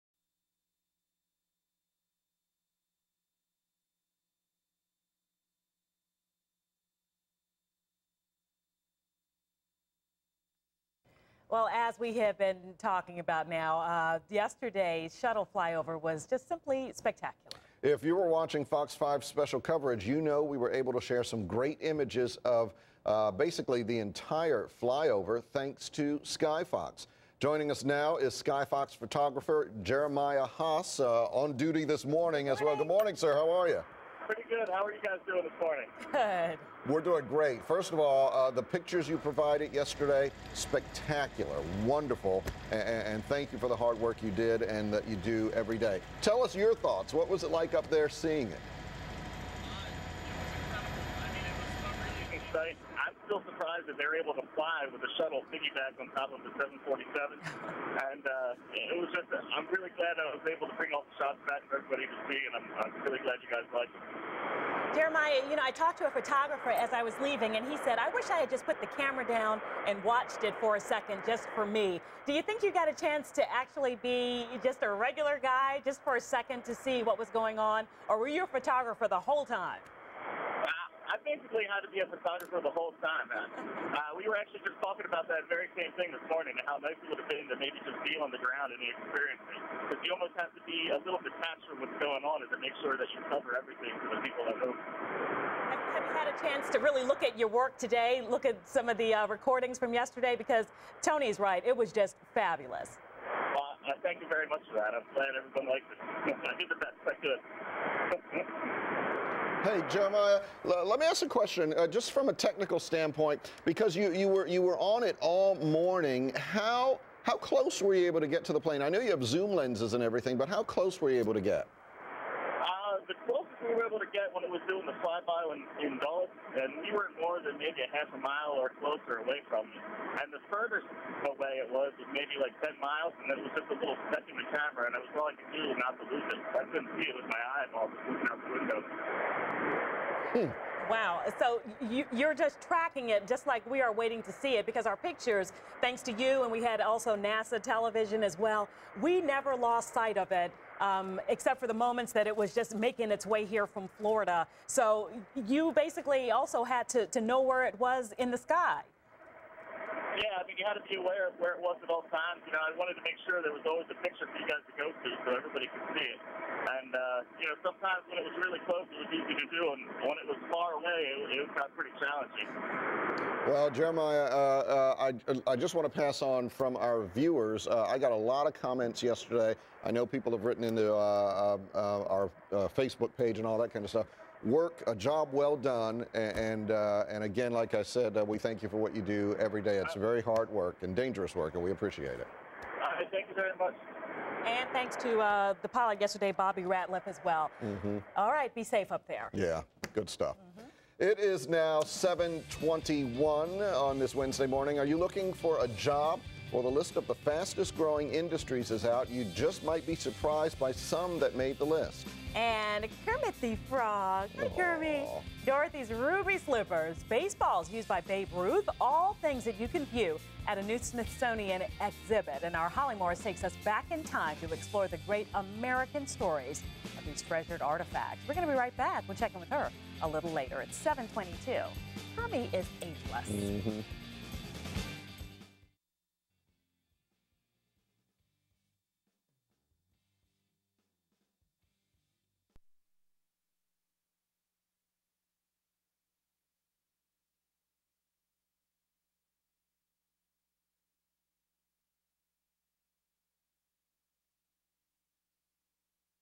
Space Shuttle Discovery Interview on FOX 5
This is a short interview I did Live on WTTG  about the Video I captured of the Space Shuttle Discovery's fly by of down town DC and landing at Dulles International.  The shuttle is now on display at National Air and Space Museum Steven F. Udvar-Hazy Center.